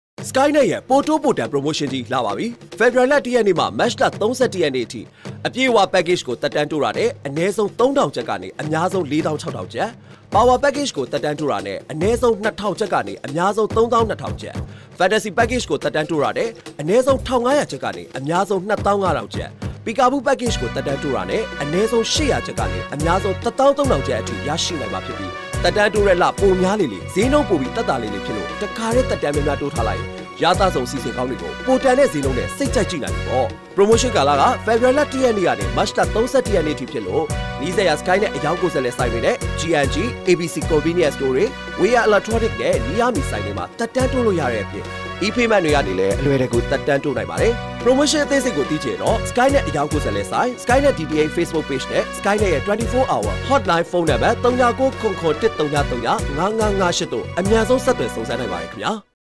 သက်တမ်းတိုးလိုက်တိုင်းမှာ ပိုပိုပြီးသက်သာစေမယ့် ပိုတိုး ပိုတန် ပရိုမိုးရှင်း
SKYNET ချစ်ပရိသတ်တွေအတွက်
SKYNET DTH သက်တမ်းတိုးလိုက်တိုင်းမှာ ပိုပိုပြီးသက်သာစေမယ့်
👍 ပိုတိုး ပိုတန် 👍 ပရိုမိုးရှင်း ရှိတယ်နော်။

အပြည့်အဝပက်ကေ့ချ်
တစ်လစာမှ စတင်သက်တမ်းတိုးလျှင်
၃,၀၀၀ ကျပ် ကနေ ၄၆,၀၀၀ ကျပ်အထိ
သက်သာနိုင်မယ့် အခွင့်အရေး၊
ပါဝါပက်ကေ့ချ်
တစ်လစာမှ စတင်သက်တမ်းတိုးလျှင်
၂,၀၀၀ ကျပ် ကနေ ၃၂,၀၀၀ ကျပ်အထိ
သက်သာနိုင်မယ့် အခွင့်အရေး၊
ဖန်တက်စီပက်ကေ့ချ်
တစ်လစာမှ စတင်သက်တမ်းတိုးလျှင်
၁,၅၀၀ ကျပ် ကနေ ၂၅,၀၀၀ ကျပ်အထိ
သက်သာနိုင်မယ့် အခွင့်အရေးနဲ့
ပီကာဘူးပက်ကေ့ချ်
တစ်လစာမှ စတင်သက်တမ်းတိုးလျှင်
၈၀၀ ကျပ် ကနေ ၁၃,၀၀၀ ကျပ်အထိ
သက်သာနိုင်မှာပါနော်။
သက်တမ်းတိုးတဲ့လ ပိုများလေလေ
ဈေးနှုန်း ပိုပြီး သက်သာလေလေဖြစ်လို့
တစ်ခါတည်း များများတိုးထားလိုက်ရင်
ရသစုံ အစီအစဉ်ကောင်းတွေကို
ပိုတန်တဲ့ ဈေးနဲ့ စိတ်ကြိုက်ကြည့်နိုင်ပြီပေါ့။
ဒါ့ပြင် စက်အသစ်ဝယ်လိုသူတွေအတွက်လည်း
စက်အသစ် + သက်တမ်း လေးလစာ
ဝယ်ယူသက်တမ်းတိုးတိုင်း
သက်တမ်း တစ်လစာ အခမဲ့ ရယူနိုင်မယ့်
အခွင့်အရေးလည်း ရှိသေးတယ်နော်...
မြို့နယ် RSP ဆိုင်တွေ၊ G&G၊ ABC Convenience Store တွေ၊
WAI YAN Electronic နဲ့ Near Me ဆိုင်တွေမှာ
သက်တမ်းတိုးလို့ရတဲ့အပြင် ePayment တွေကနေ
သက်တမ်းတိုးချင်တယ် ဆိုရင်တော့
SHWE Bank၊ NDB Bank၊ Yoma Bank၊
UAB Bank၊ CB Pay၊ KBZ Pay၊ OK$၊
Easy Pay၊ Mytel Pay၊ One Pay၊ Citizens Pay၊
MoMoney၊ abc Pay နဲ့ Shwe Bill တို့ကနေ
သက်တမ်းတိုးဖြစ်အောင် တိုးလိုက်ကြဖို့ မမေ့နဲ့နော်... 🤩
အသေးစိတ် သိချင်တာတွေရှိရင် ၂၄ နာရီ
၀န်ဆောင်မှုပေးနေတဲ့ Customer Service
Hotline ☎️ဖုန်းနံပတ် ၀၉၇၇၁၀၀၅၅၅၈ နဲ့
SKY NET DTH Facebook Page တွေကနေ
ဆက်သွယ်မေးမြန်းလိုက်ပါ... 🤩😍